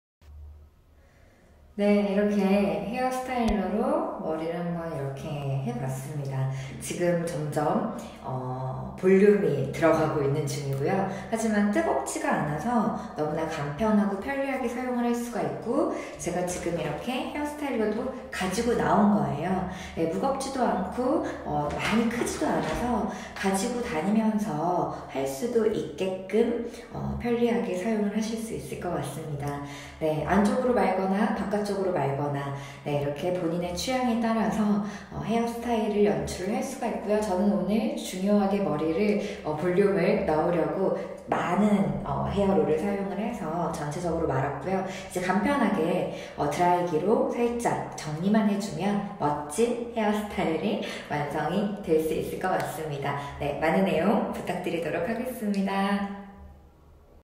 💩🤚➡️금손이 될수있는 헤어스타일러, 19년 3월 속리산 | 애터미 나리의 브이로그 Nari’s Vlog
헤어스타일러 사용하는것 잠깐 찍어보았던 영상,
업로드 해봅니다😊
헤어스타일러 사용법 어렵지 않아요.
정말 간편하면서도, 헤어샵에서 머리한 것 같다는 말 들을 수 있어요😆